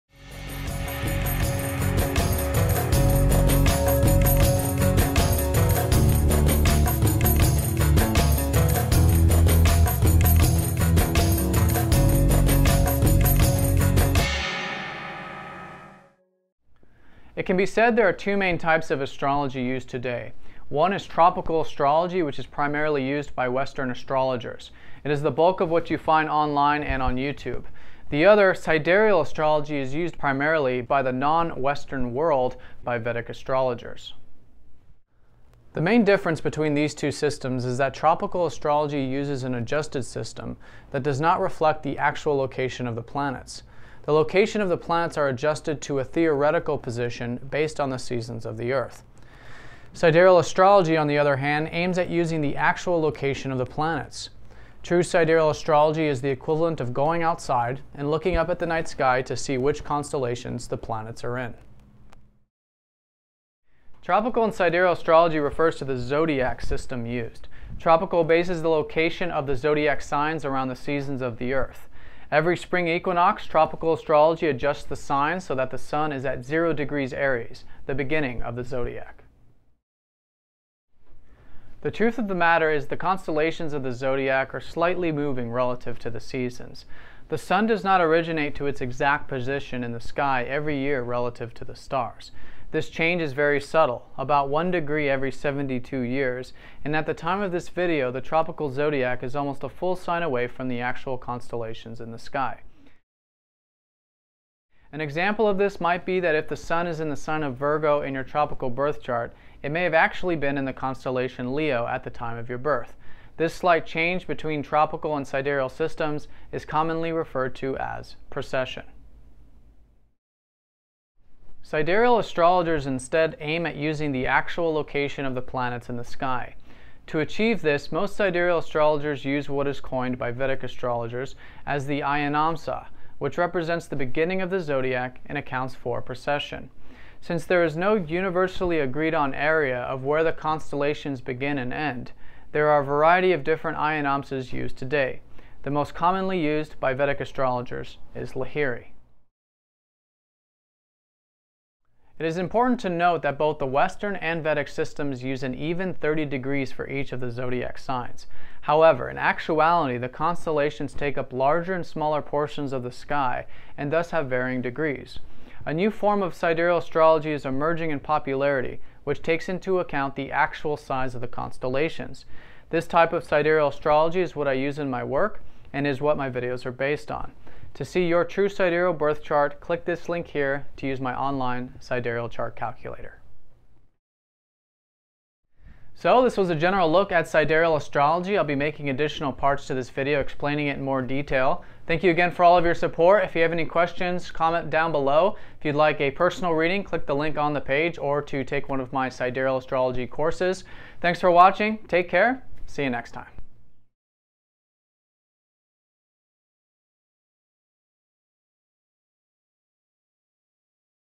What is Sidereal Astrology?
This video covers what sidereal astrology is and the difference between tropical vs sidereal astrology. Tropical astrology is what we most commonly find online. However, most don't know that this system does not use the actual location of the planets in the sky. Instead it uses an adjusted system based on the seasons of the Earth, and it is currently almost a full sign away from the actual constellations in the sky. Sidereal astrology, on the other hand, uses the true location of the planets in the sky.

To find out your true sidereal sign use the online sidereal chart calculator